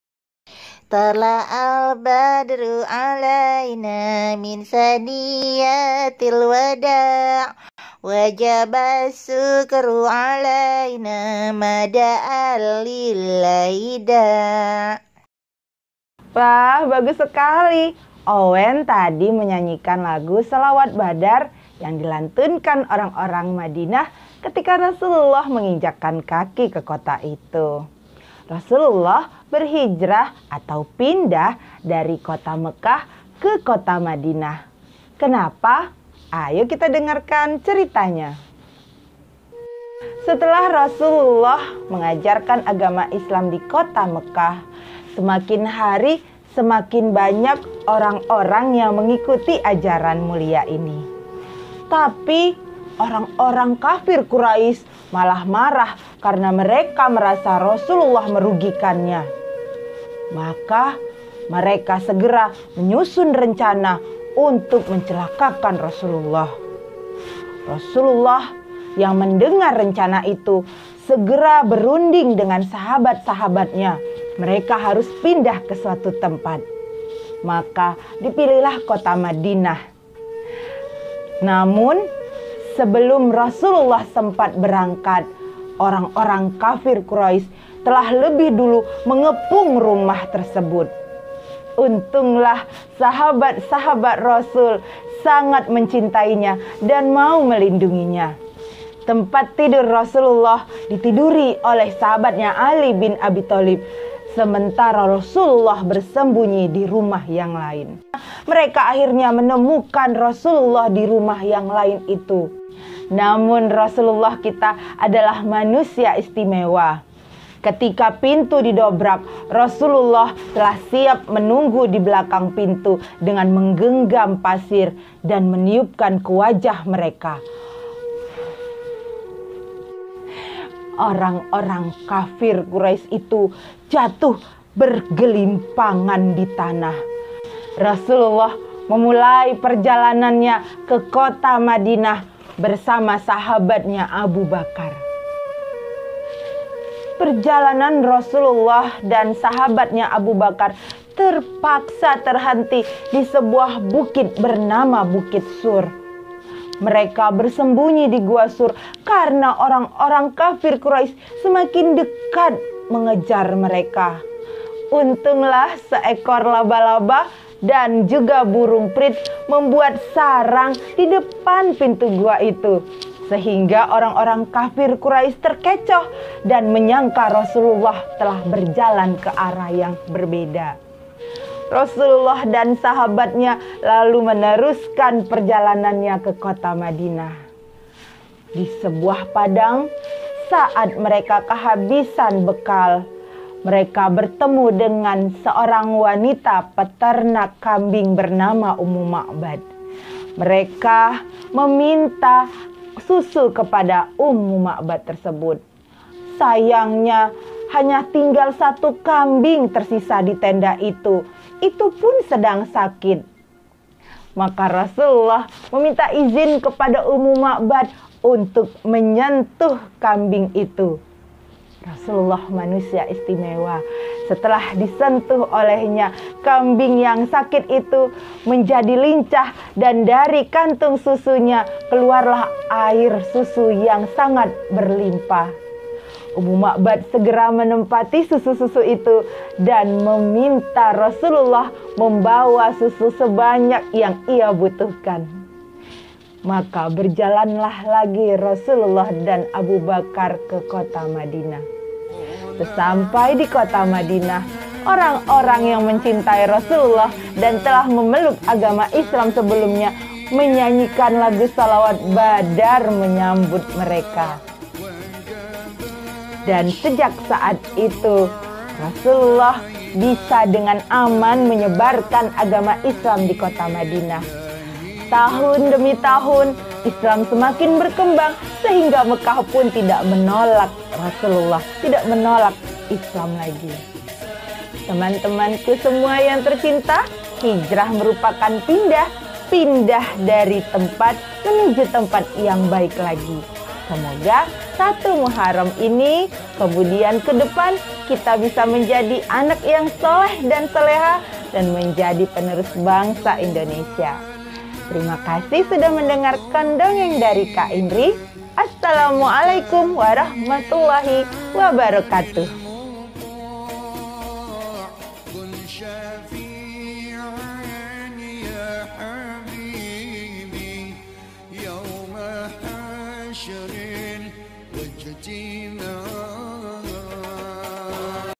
Dongeng Ramadhan
Hai adik-adik,
Ramadhan sebentar lagi datang, ya. Jaga kesehatan kalian agar bisa menyambutnya. Video ini dibuat tahun lalu saat sebuah sekolah meminta kak Indri mengisi kegiatan ramadhan secara online.

Sekarang Kak Indri akan membaginya untuk kalian.